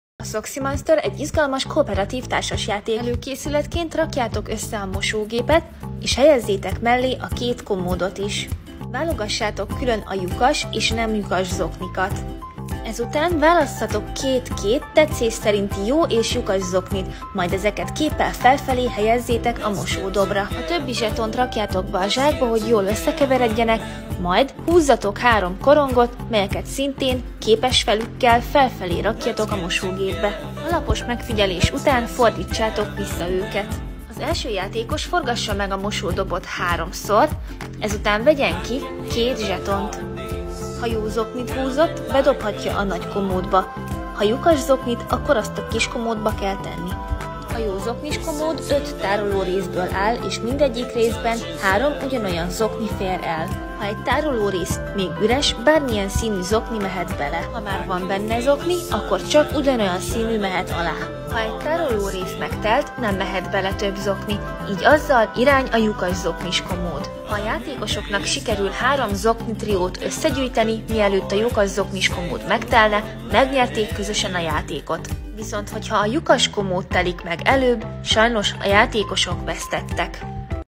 Socksy Monster - Gekkotoys.hu
A háromlábú szörnyek mosónapot tartanak, és össze kell illeszteniük a hármas zoknipárokat.

Élményt adó, tartós játékok.  Djeco márka által létrehozott termékeken neves grafikusok és illusztrátorok dolgoznak, hogy a játék élmény legyen, egyedi, stílusos és utánozhatatlan.